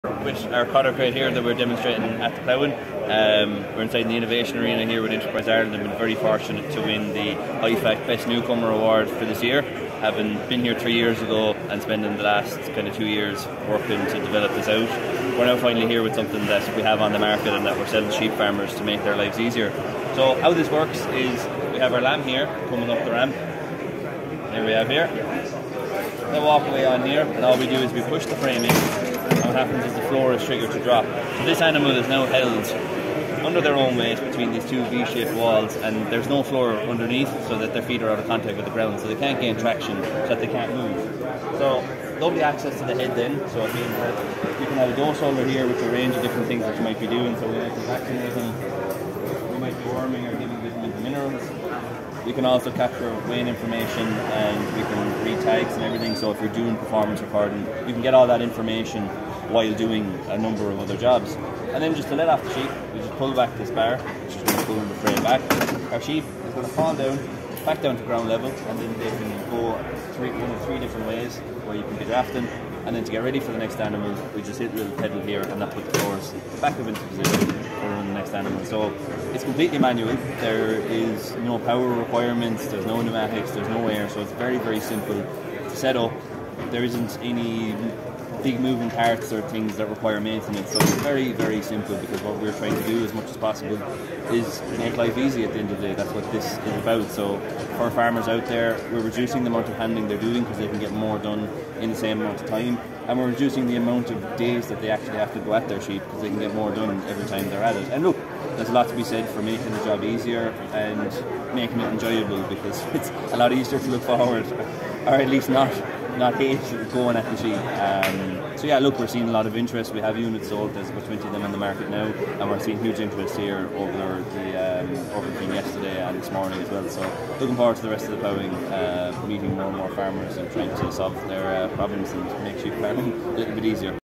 Nick Cotter from the Abbeyfeale based Cotter Crate company
Nick Cotter from the Abbeyfeale based Cotter Crate company spoke to Sari Houlihan. Their innovative sheep handling system makes handling sheep easier, faster and safer. He was at the Innovation tent at the National Ploughing Championships. The company have won numerous awards and most recently won the IFAC Best New Comer Award.